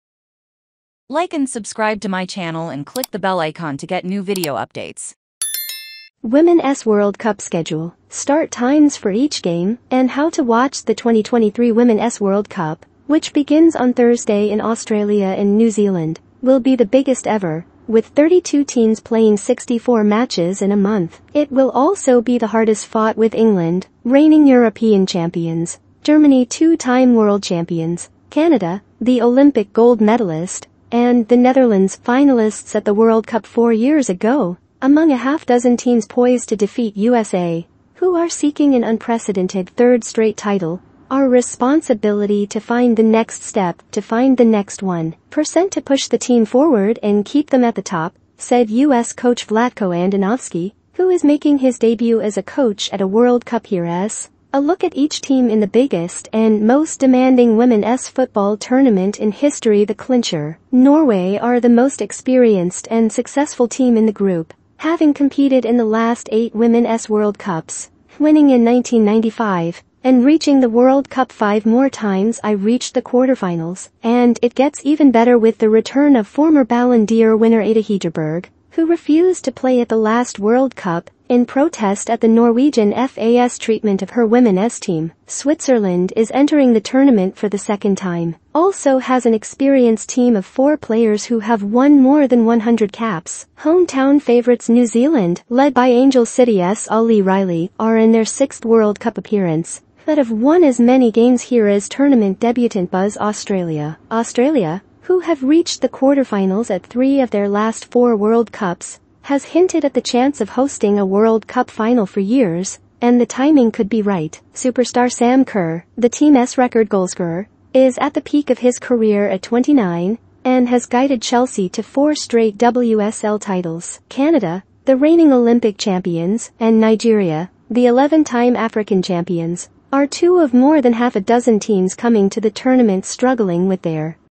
WOMENS WORLD CUP SCHEDULE: START TIMES FOR EACH GAME AND HOW TO WATCH
The 2023 Womens World Cup. which begins Thursday in Australia and New Zealand. will be the biggest ever as 32 teams play 64 matches in a month. But while Vilda remains. 11 players including MF Patri Guijarro. D Mapi León and GK Sandra Paños. who have all played for Spain more than 50 times. will miss the World Cup. weakening a side that were considered contenders. but she will be without F Vivianne Miedema. the teams all time leading scorer. who is also out with a cruciate ligament injury. The USA. who have 14 players making their World Cup debuts. are unbeaten this year and have won their last nine straight wins. eight of them against World Cup qualifiers.